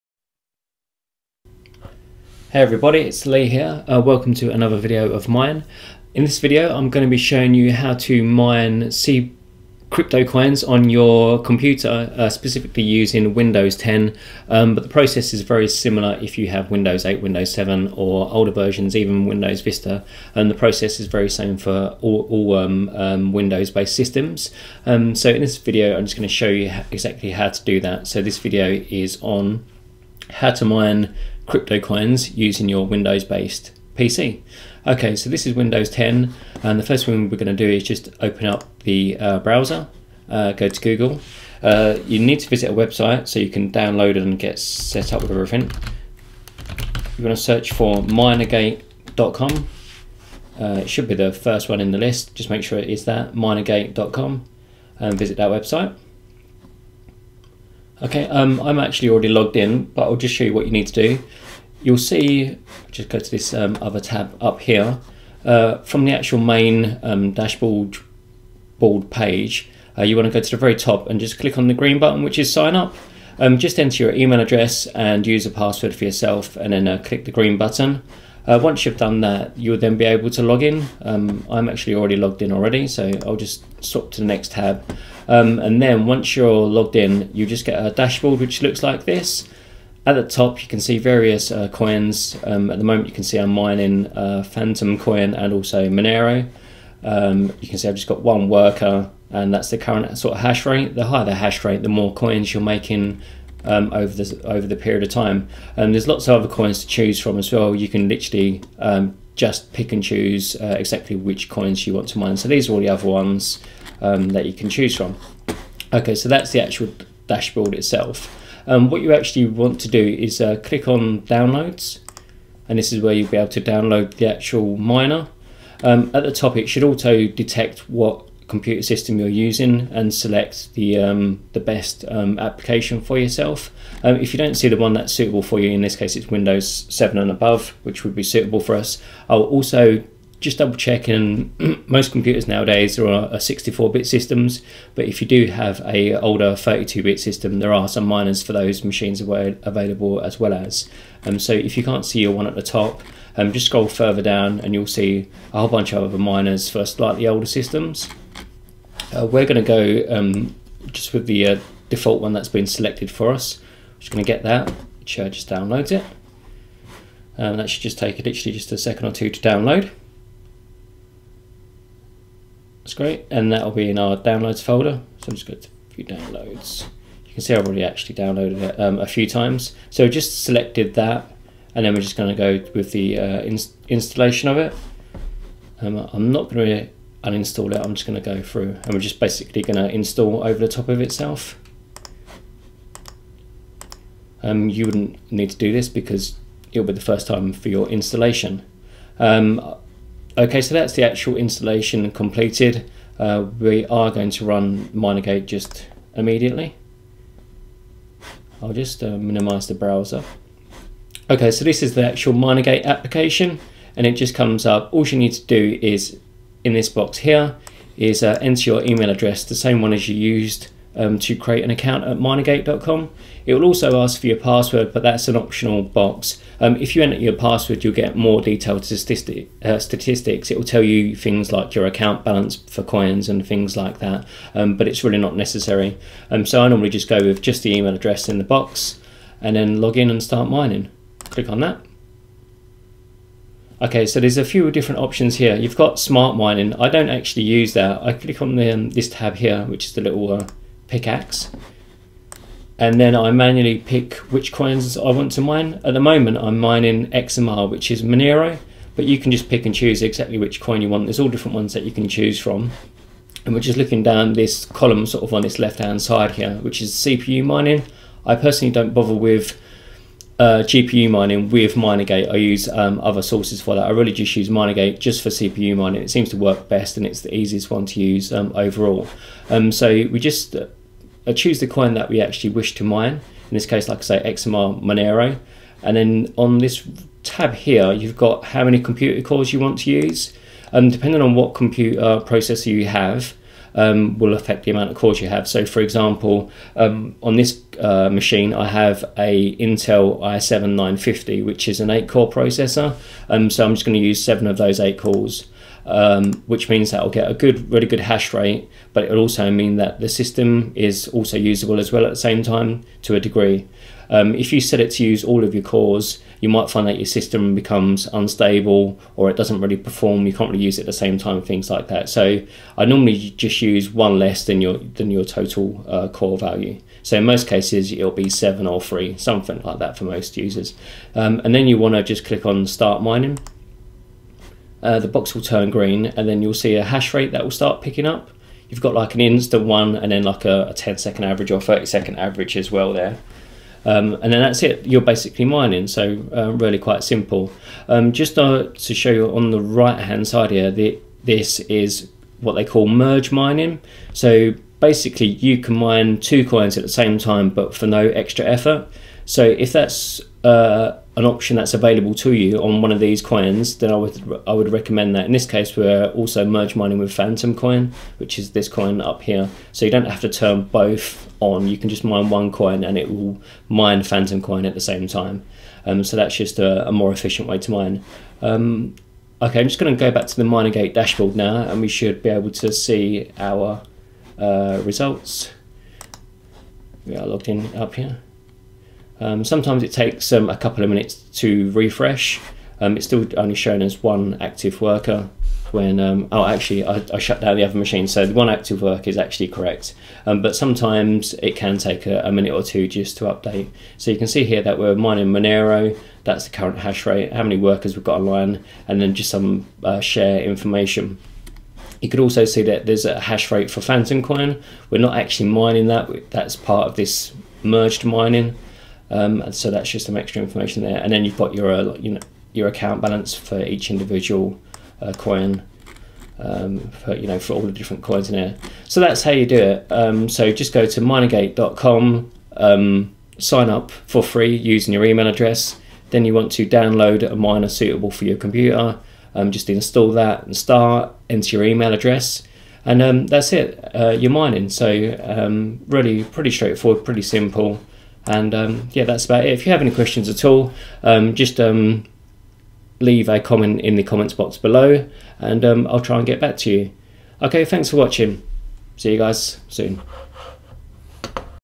How to CPU mine bitcoin like crypto coins on Windows 10
A little video I made showing how easy it is to mine crypto coins using your windows 10 computer. The process is very similar for older versions of windows too.

Good if you want to mine Monero, Aeon, Fantom, Bytecoin and many other coins.

Bitcoin 12v4d15AuopsVzudct7J8LJMm7qH95SxA1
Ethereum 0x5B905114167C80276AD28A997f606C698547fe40